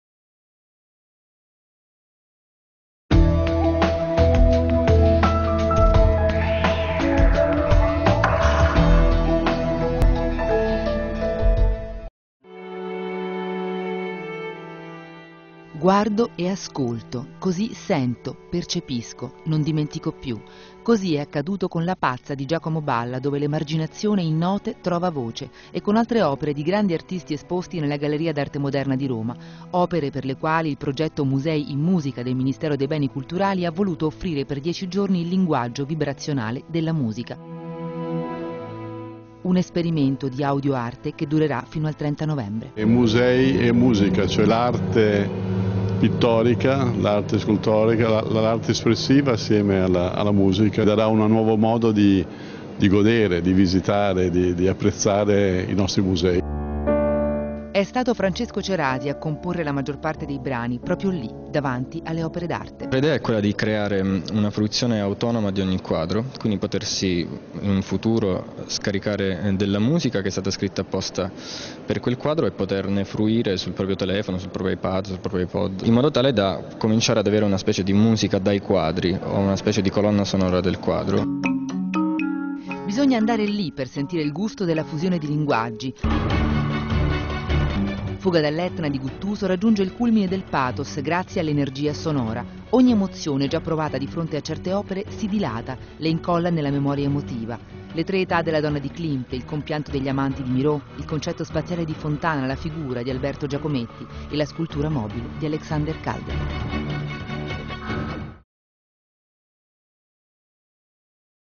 Audioarte - GNAM Rethink
Per il progetto 'musei in musica' le opere di sei grandi artisti parlano in note fino al 30 novembre 2010